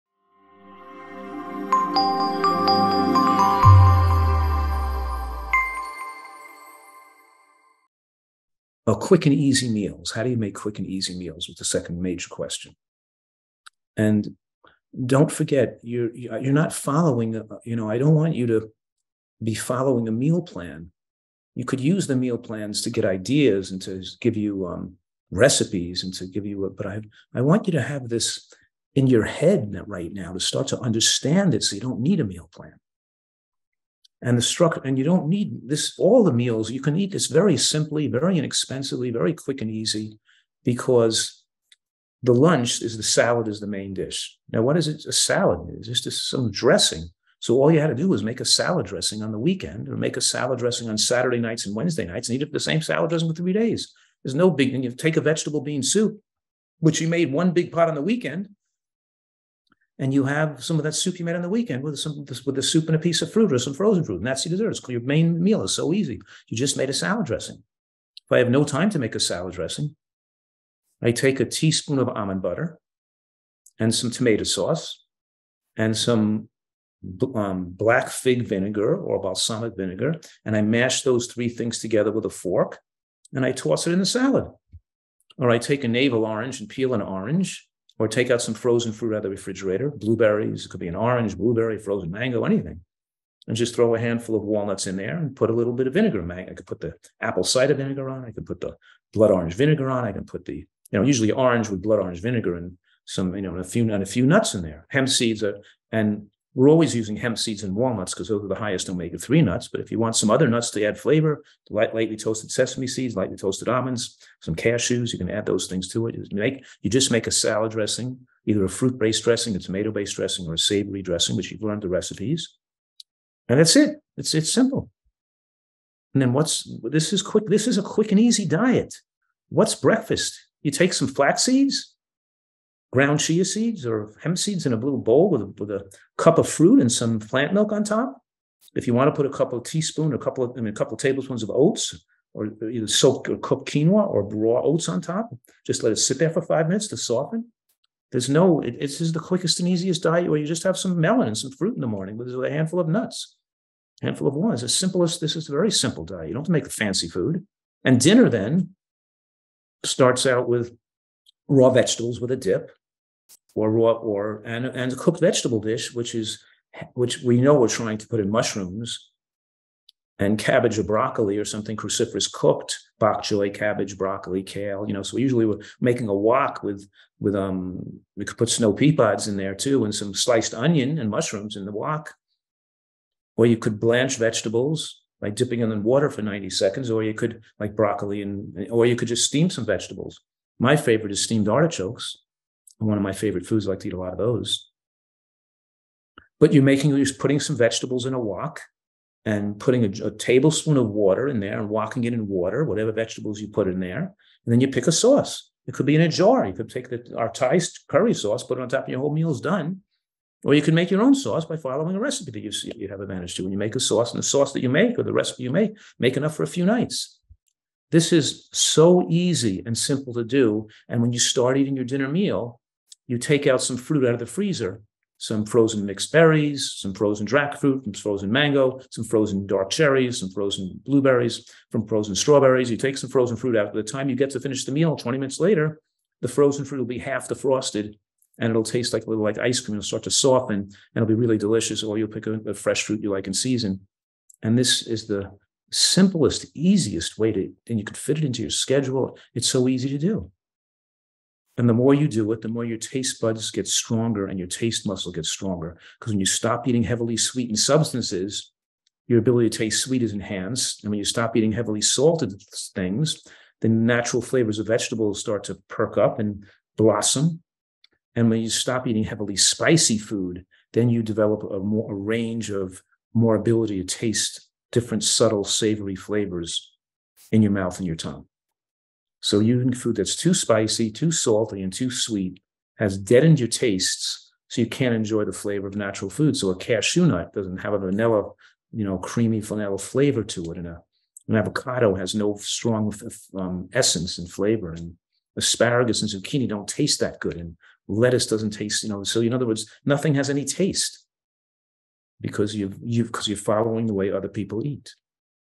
How to Make Quick and Easy Meals | Nutritarian Diet | Dr. Joel Fuhrman
Are you tired of following complicated meal plans and recipes? Do you want to eat healthy and delicious food without spending too much time in the kitchen? Well, Dr. Joel Fuhrman has tips for you.

No more meal plans or structured diets, just simple and inexpensive meals that you can make in no time. From making salads to soups to vegetable dishes, Dr. Joel has solutions for your everyday meal needs.

Plus, discover how you can enhance your taste buds and enjoy the natural flavors of food by avoiding heavily sweetened, salty, and spicy foods.

Get ready to simplify your diet and enjoy delicious, healthy meals every day!